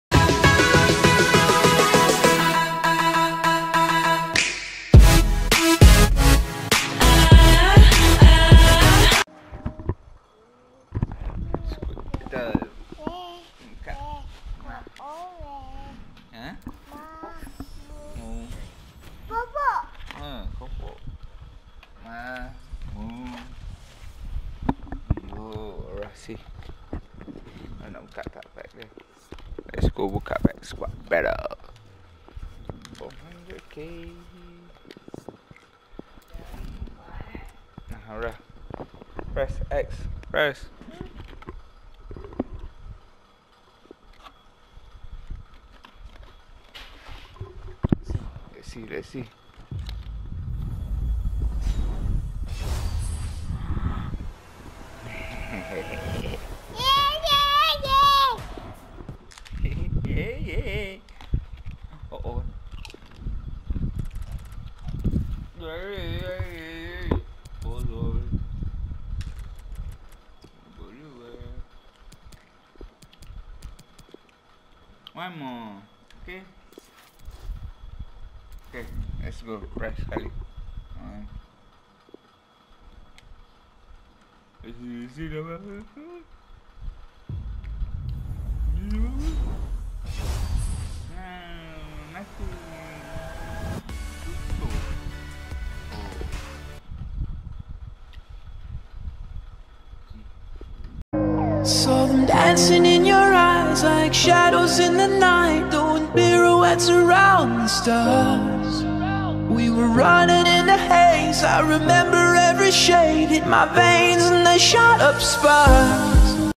Last time go for GOLD 2!!! #7
Ingatkan gold 2 punya rewards walaupun sikit lagi padu , salah teori ni nampaknya , we will got for elite 3 again ah , byk skit pack